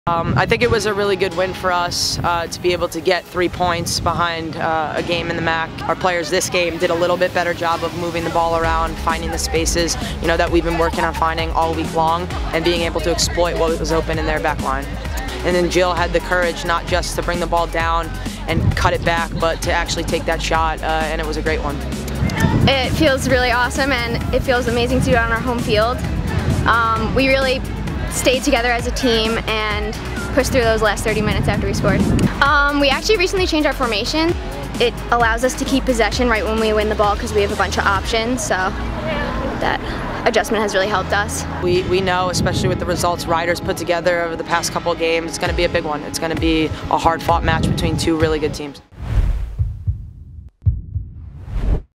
Women's Soccer Downs Quinnipiac for First MAAC Win
Women's soccer gained their first MAAC win at home, as Jillian Colucci struck in the game winning goal!